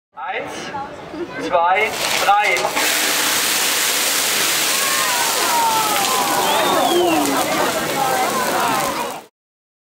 Feuerwehr Nürnberg Fettexplosion
Feuerwehr Nürnberg Fettexplosion. So sieht das aus, wenn man einen Fettbrand mit Wasser löscht. Hier bei einer Vorführung der Nürnberger Berufsfeuerwehr.